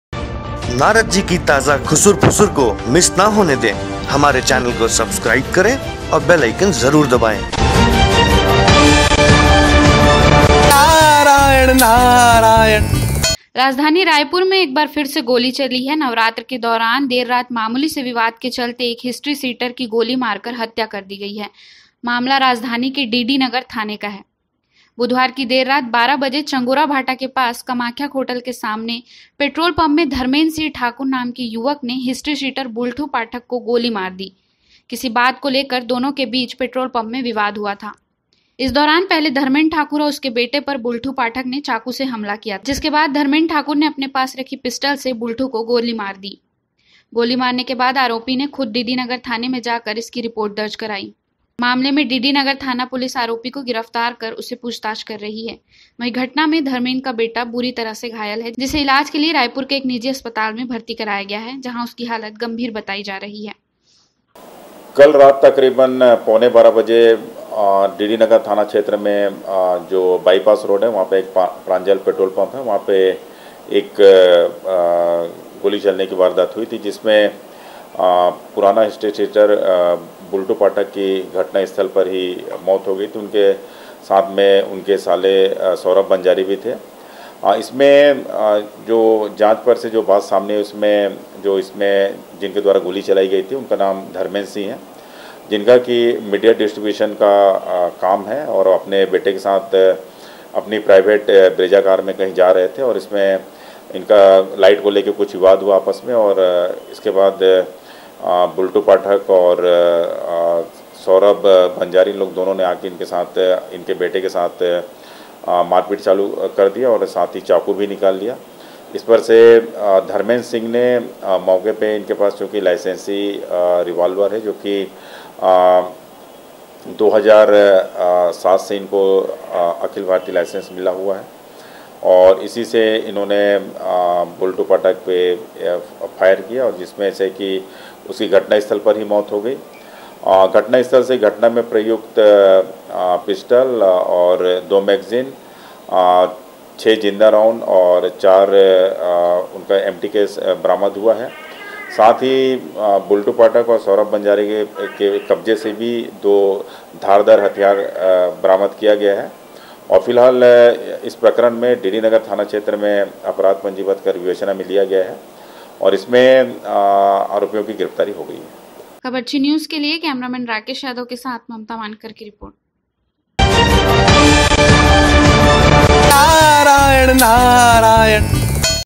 CCTV में कैद हुआ पेट्रोल पम्प का गोलीकांड, मौके पर मौत
राजधानी रायपुर में देर रात मामूली से विवाद के चलते एक हिस्ट्री शीटर की गोली मारकर हत्या कर दी गई है। मामला राजधानी के डीडी नगर थाने का है। बुधवार की देर रात 12 बजे चंगोराभाठा के कामाख्या होटल के सामने पेट्रोल पंप में धर्मेंद्र सिंह ठाकुर नाम के युवक ने हिस्ट्रीशीटर बुलठू पाठक को गोली मार दी है। किसी बात को लेकर दोनों के बीच पेट्रोल पंप मे विवाद हुआ। इस दौरान पहले धर्मेंद्र ठाकुर और उसके पुत्र पर बुलठू पाठक ने चाकू से हमला किया। जिसके बाद धर्मेंद्र ठाकुर नेे अपने पास रखी पिस्टल से बुलठू को गोली मार दी है। गोली मारने के बाद आरोपी खुद डीडी नगर थाने जाकर इसकी रिपोर्ट दर्ज कराई है। मामले में डीडी नगर थाना पुलिस आरोपी को गिरफ्तार कर उससे पूछताछ कर रही है | वहीं घटना में धर्मेंद्र का बेटा बुरी तरह से घायल है जिसे इलाज के लिए रायपुरा के ओम हॉस्पिटल में भर्ती कराया गया है। जहां उसकी हालत गंभीर बताई जा रही है।